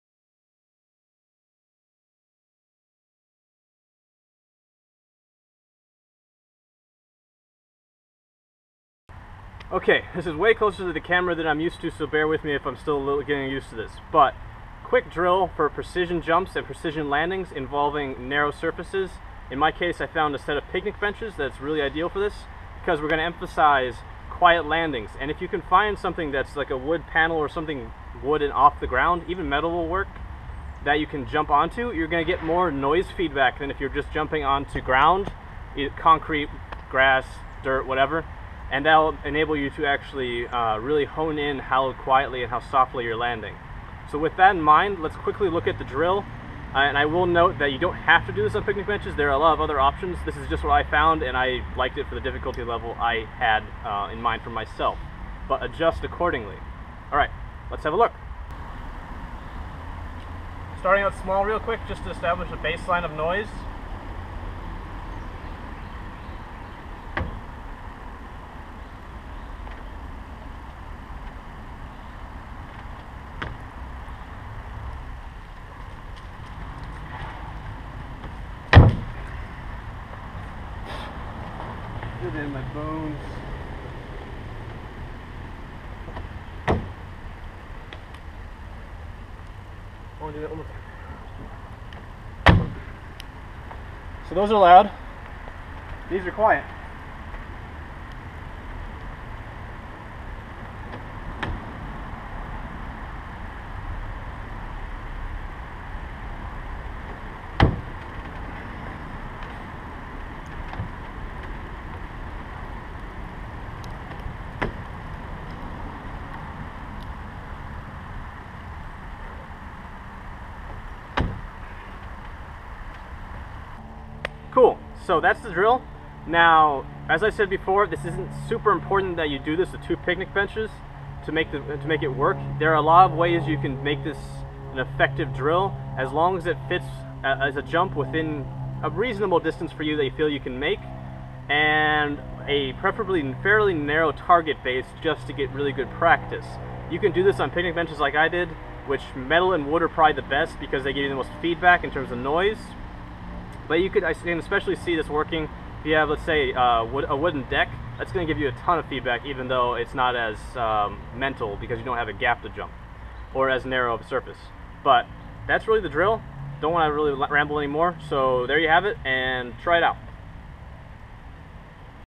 Quiet Precision Landings Drill (with Park Benches)
Quiet landings are a sign of good landing mechanics and sustainable training practices. There are many ways to sense the impact levels of a landing, but sound is often the easiest and most objective to perceive. Try it out!